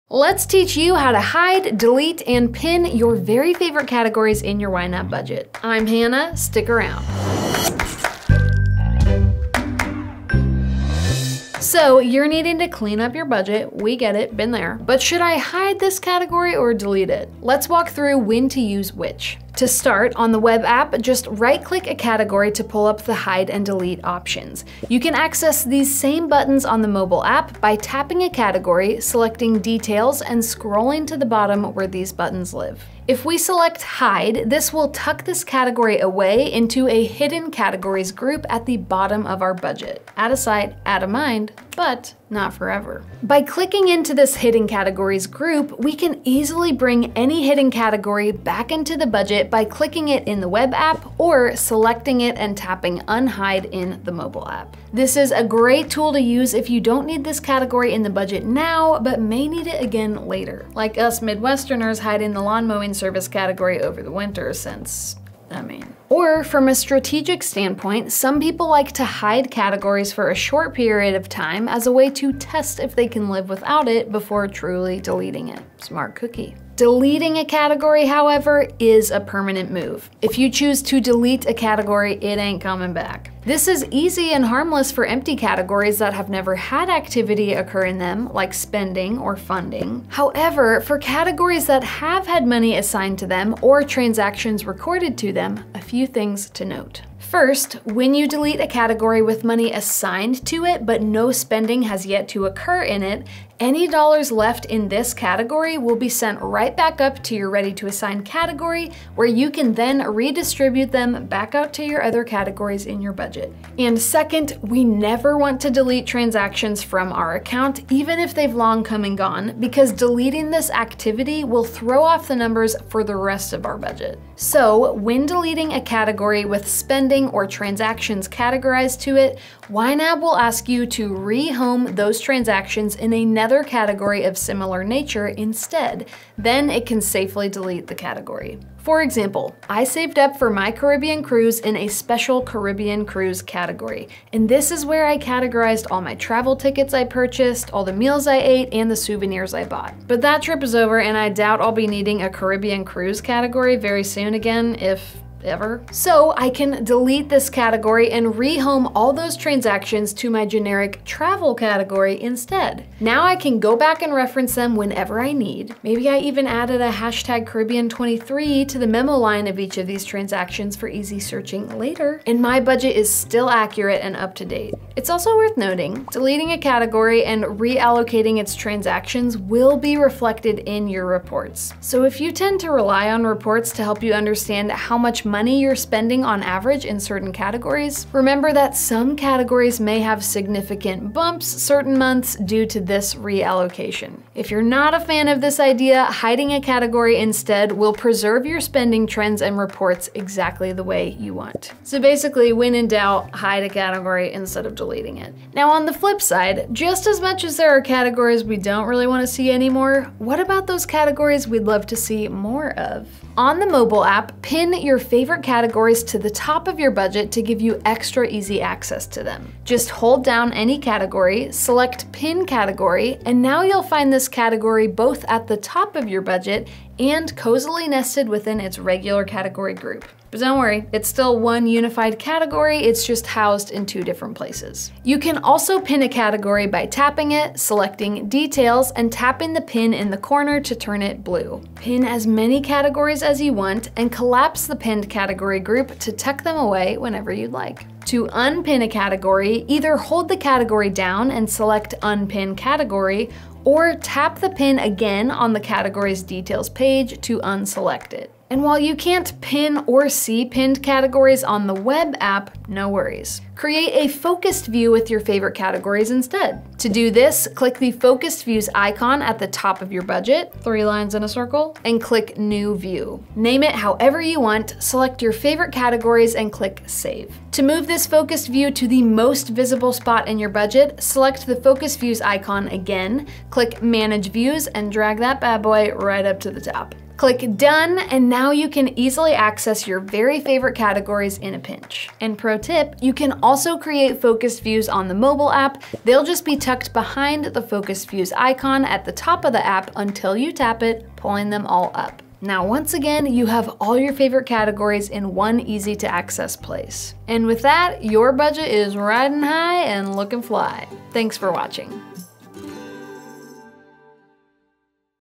YNAB Quick & Simple: Hiding, Deleting, and Pinning Categories
Looking for a no-nonsense tutorial on how to hide, delete, and pin categories in YNAB's mobile and web apps? You've come to the right place! Click the timestamps below or use the progress bar to navigate to the exact section you're looking for to get your pressing questions answered in five minutes or less!

TIMESTAMPS:
0:00 Intro
0:20 Hiding Categories on Web/Mobile
1:16 Deleting Categories on Web/Mobile
2:55 How Deleting Categories Affects Reports
3:25 Pinning Categories on Mobile
4:19 Focused Views on Web
4:52 Focused Views on Mobile
5:07 Outro